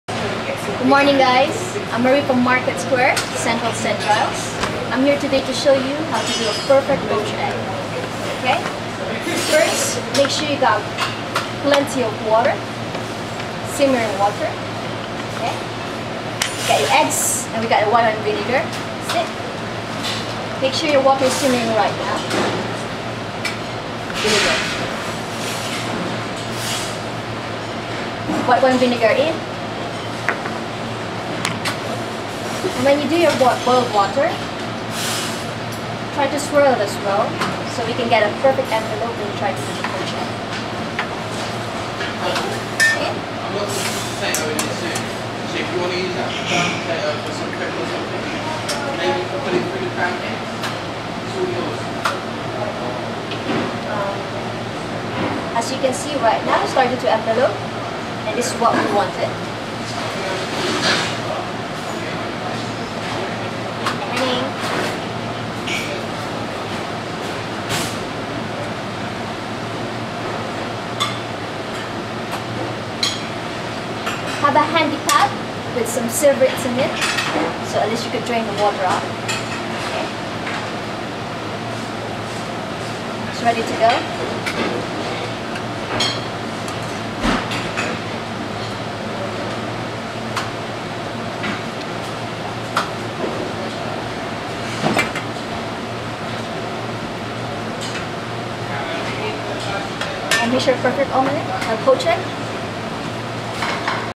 Marie poaching a perfect egg
Marie in Market Square at Central St Giles shows us how to perfectly poach an egg